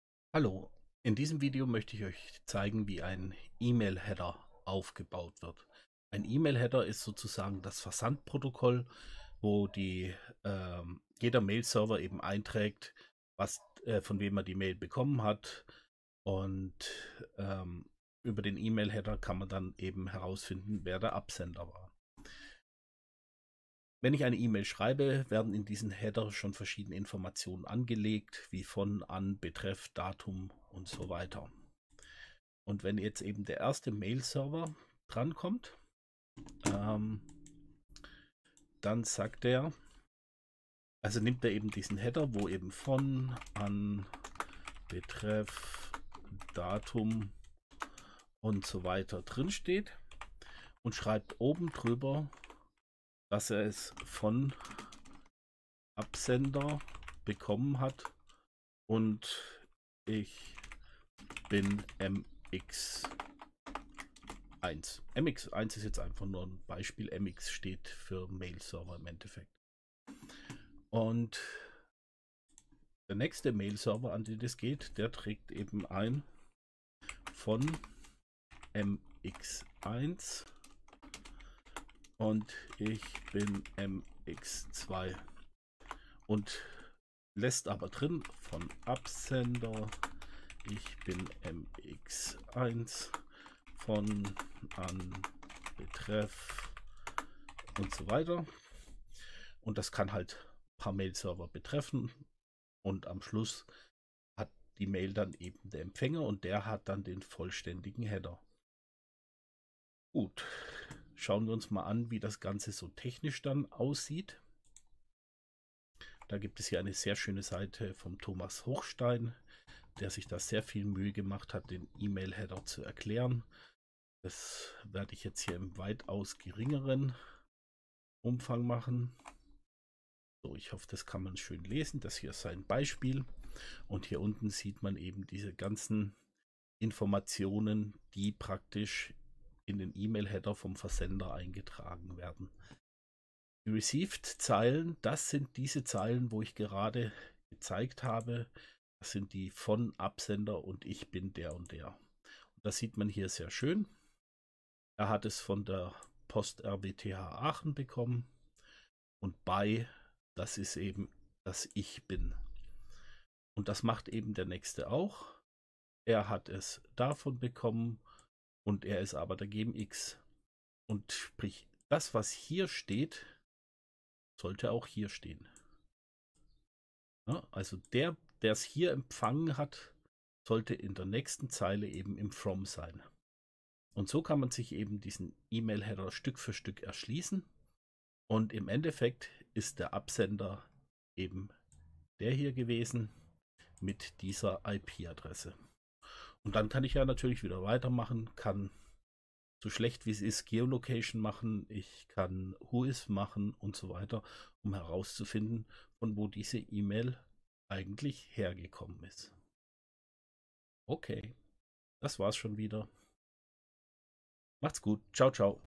#16 Wie entsteht ein E-Mail-Header - OSINT für Einsteiger
In diesem Video zeige ich euch wie ein E-Mail-Header entsteht und aufgebaut ist.